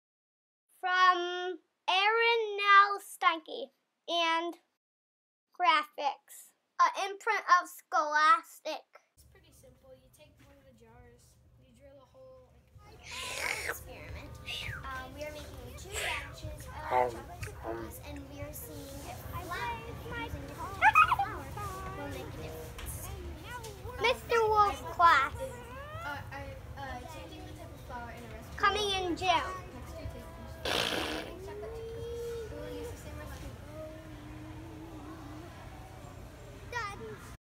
Mr. Wolf's Class by Aron Nels Steinke | Official Book Trailer
From Eisner Award-winning creator Aron Nels Steinke comes a vibrant and funny new series that charmingly captures the everyday antics of a fourth-grade classroom in Mr. Wolf’s Class.

Mr. Wolf has just started teaching at Hazelwood Elementary. He wants the first day of school to go well, but he's got his hands full with his new class. Some of his students include: Margot, who is new in town and is trying to make friends. Sampson, who brought something special to school for show-and-tell. Aziza, who just wants everyone to be quiet and do their work. And Penny, who is VERY sleepy because she has a new baby brother at home, goes missing!

This delightful new series captures the everyday and unexpected ups and downs of a fourth-grade classroom.